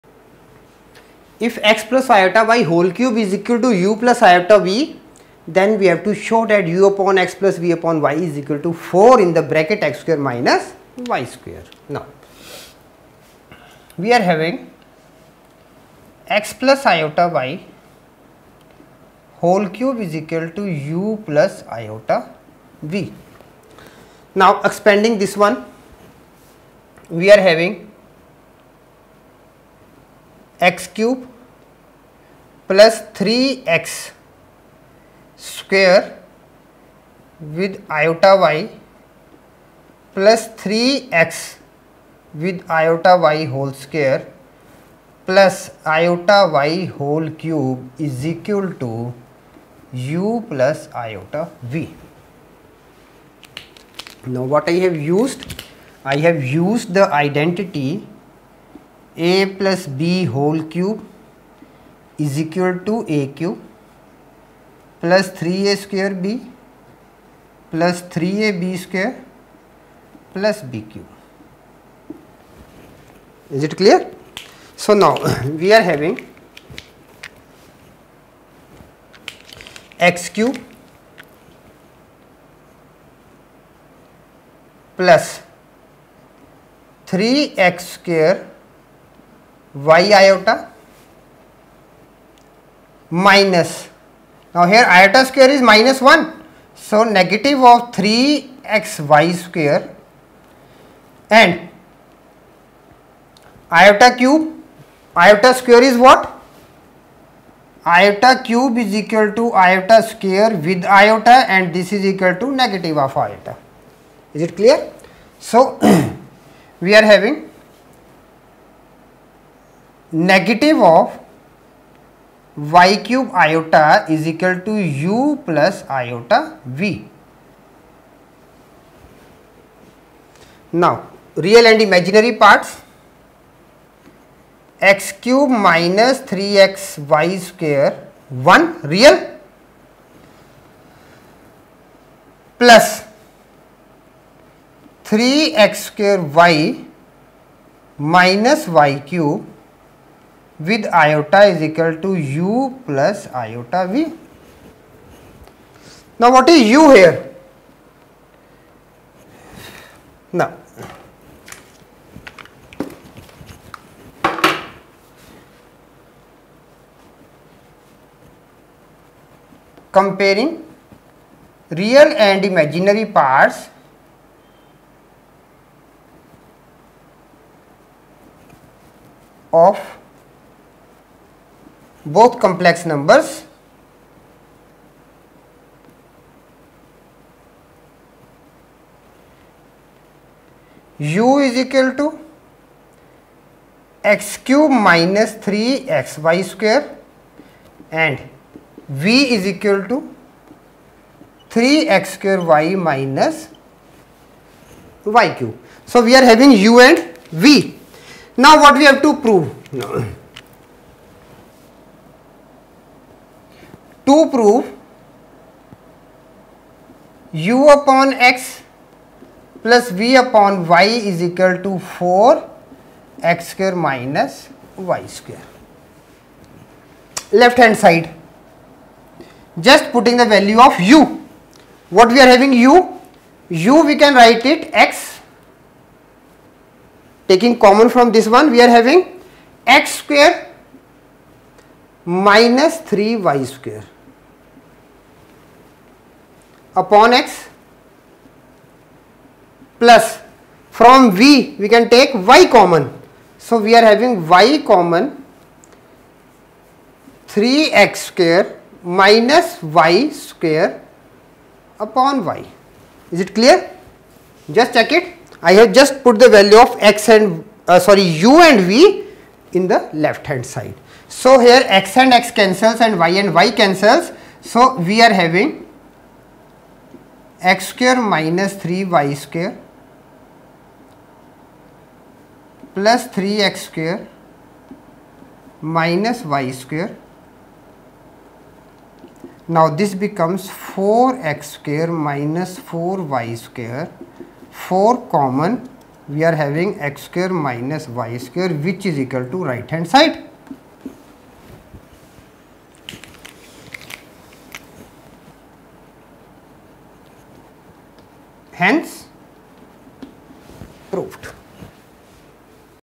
ICSE Free Topic l MATH 12 l COMPLEX NUMBER l Important Question l Complex Number l L905
Complex Numbers, ICSE Class 12 have been explained in this bullet course and each and every minute detail has been covered so that students have a thorough understanding and can deal with any question based on this topic.